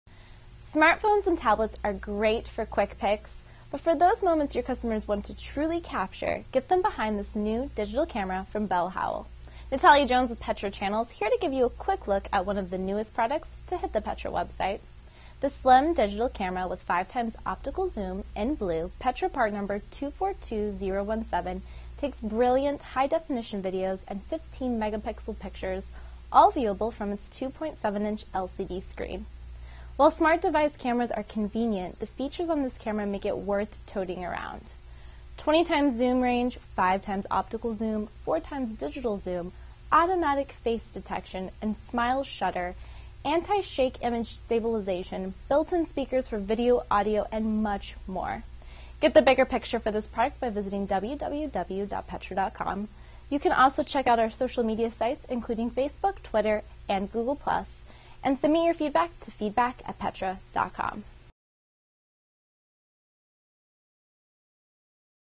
Petra Pulse - Week 6, 2014: Part 1
Get a quick snapshot of what's new In this week's Petra Pulse, Natalia gives the rundown on the new Slim Digital Camera with 5x Optical Zoom, in Blue, from Bell+Howell (Petra Part Number 242017).